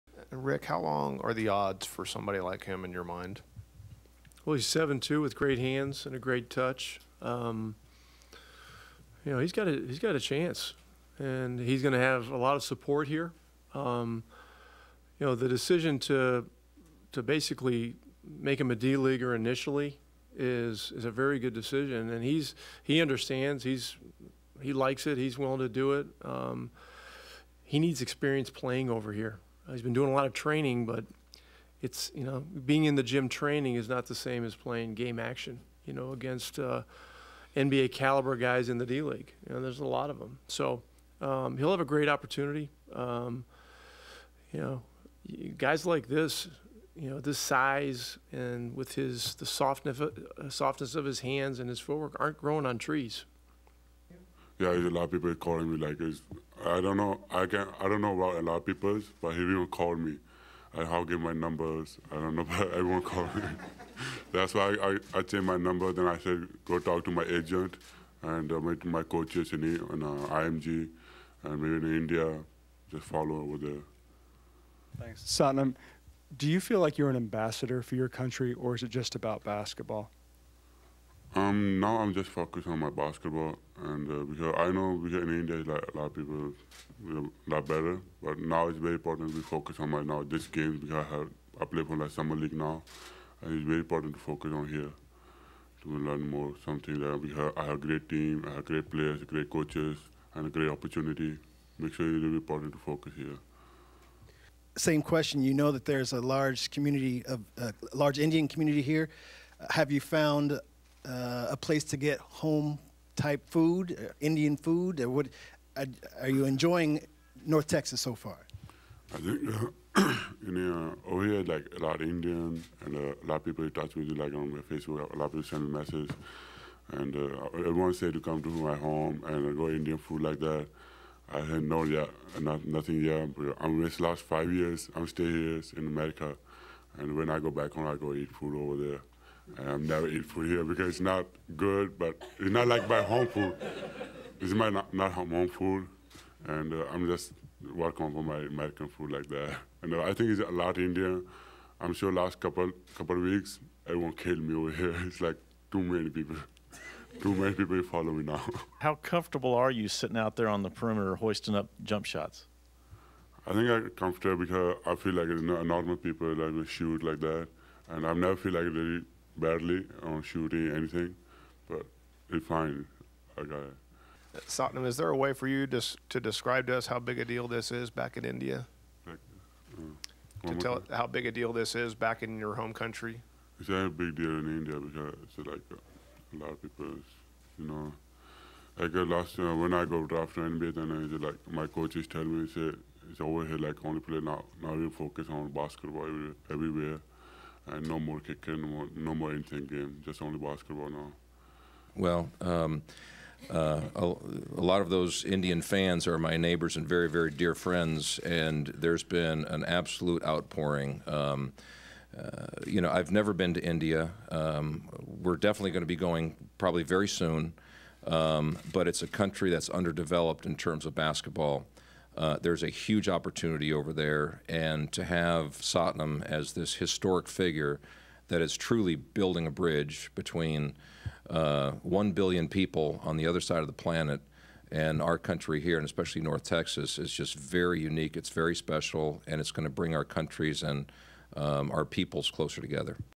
Satnam Singh Press Conference Clips
Satnam Singh addressed the media at the Mavericks Clips from the Mavs Rookie Introductory Press Conference.